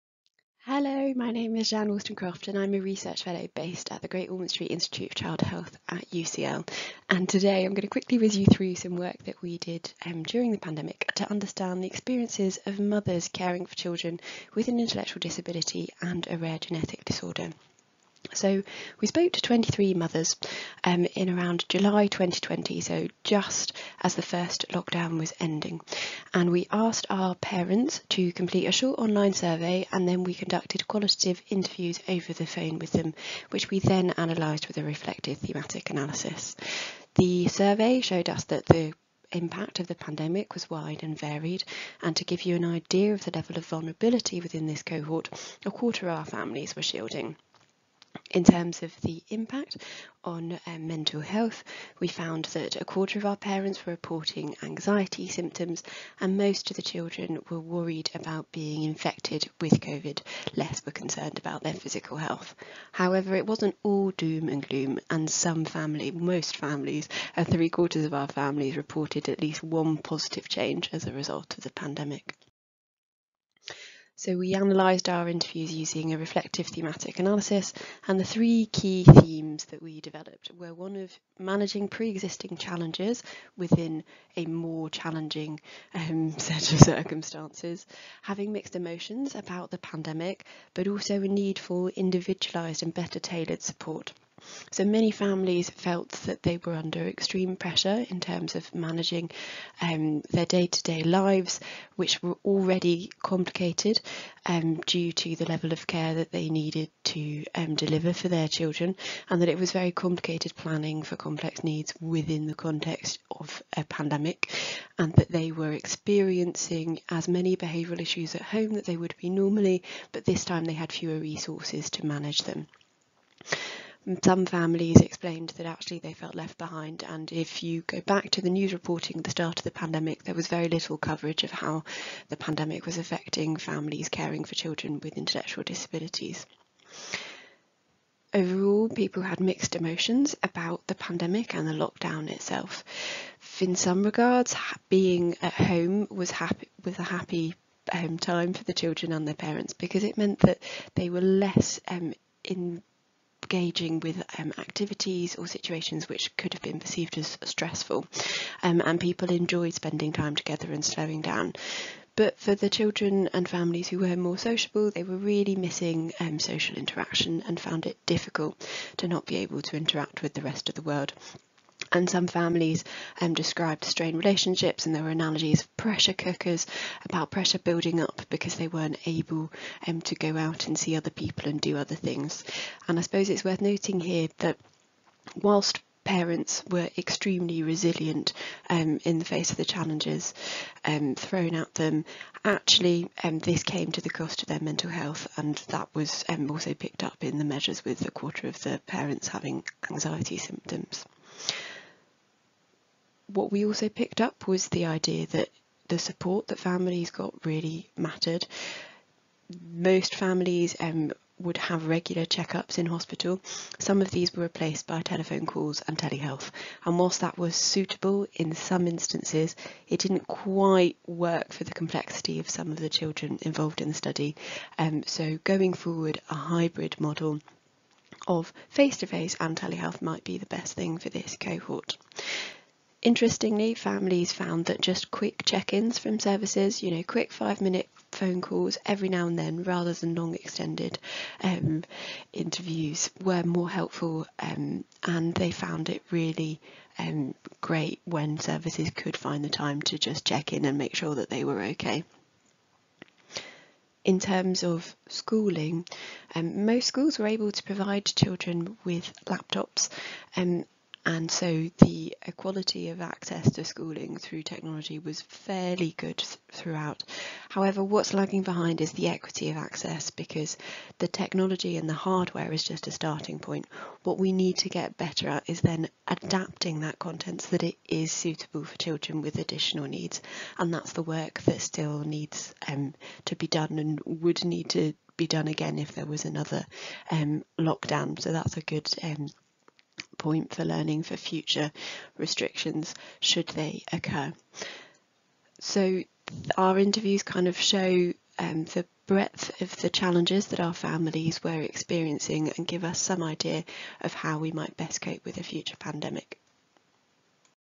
IMAGINE ID Coronavirus Experiences Study: “We’ve been in lockdown since he was born”
Description: Qualitative study (thematic analysis) exploring the experiences of families caring for children with an intellectual disability during the pandemic.